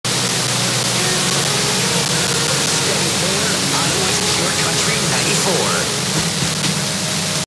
93.9 - CKKL - Ottawa, ON - Pure Country 94 (Sporadic Es)
Aircheck of 93.9  - CKKL  - Ottawa, ON - Pure Country 94  (Sporadic Es)
Received 6/28/21 @ 10:59 EDT in Charleston, SC

Antenna: Stellar Labs 4-element FM Yagi antenna
Rotor - RCA VH226E Antenna Rotor

Visit DX Central for more DX tips, airchecks and educational resources for the AM and FM Dxer
"DX Central - because we're all about radio!"